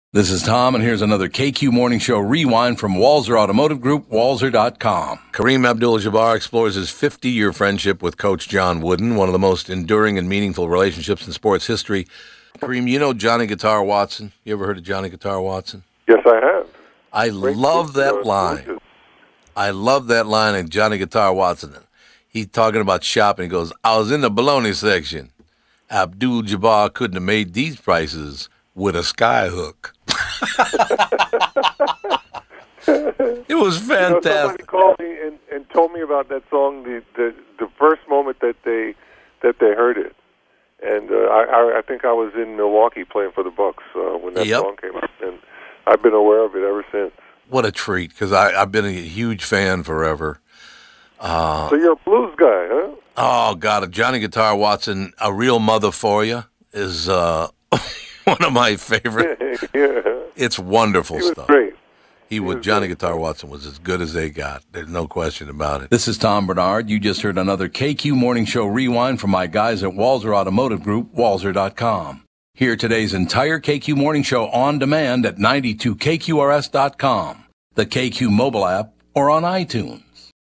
Kareem Abdul Jabbar
Kareen Abdul-Jabbar's new book Coach Wooden and Me: Our 50-Year Friendship On and Off the Court is available now.